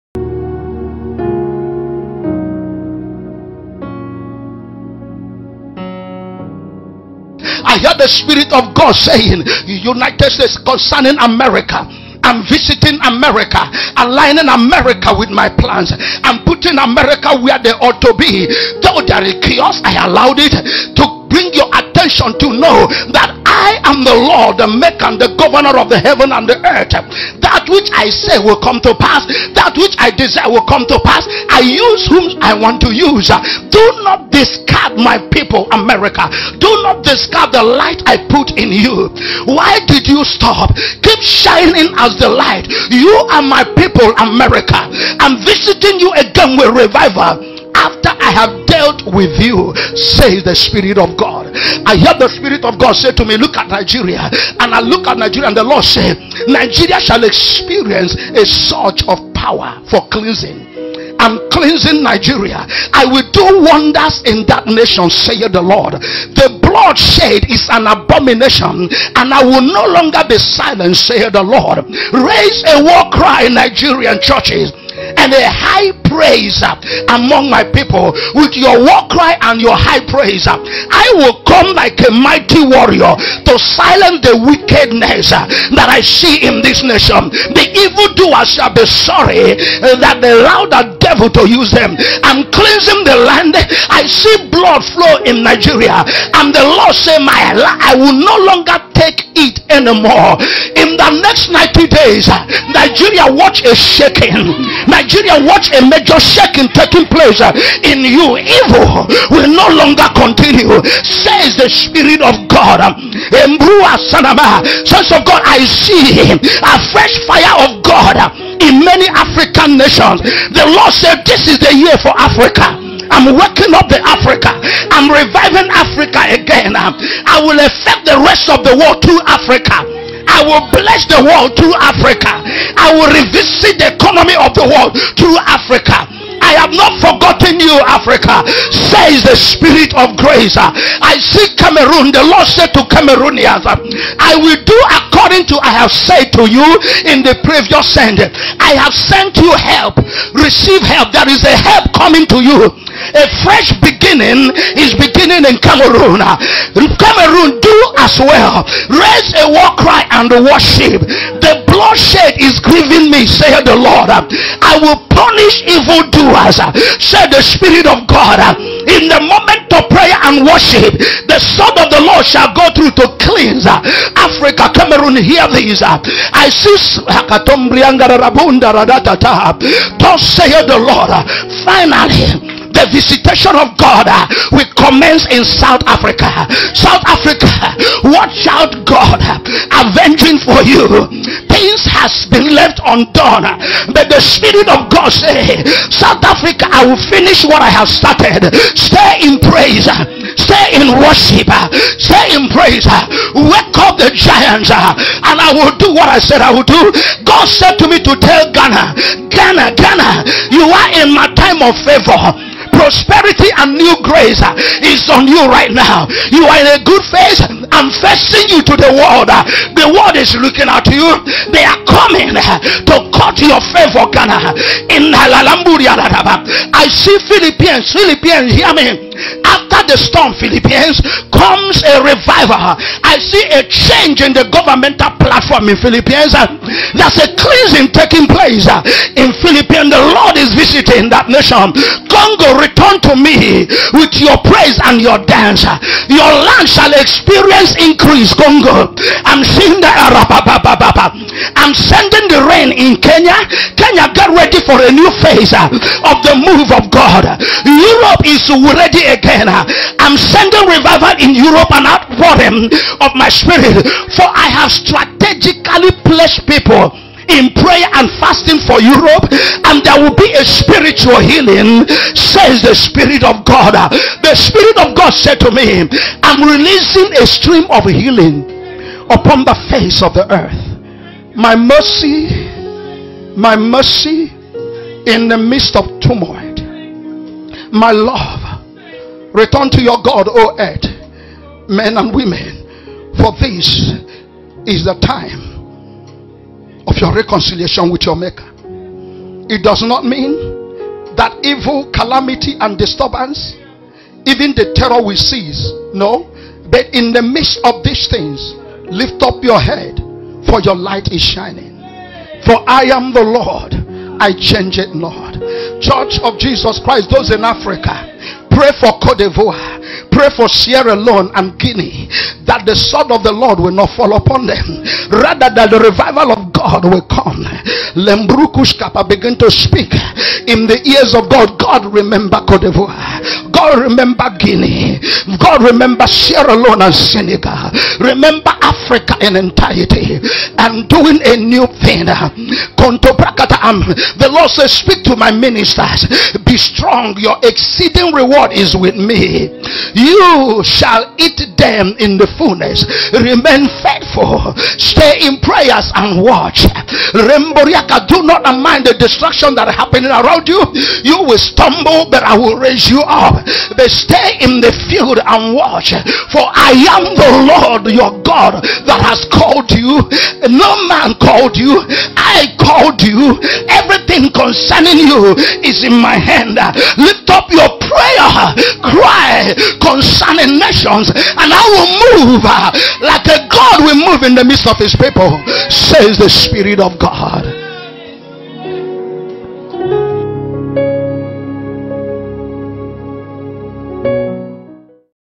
2020 Prophecy For the Nations- Bishop Joel Njoku-Andrew
Bishop Joel Njoku-Andrew prophecies over the nations as instructed by the Holy Spirit! Watch and share!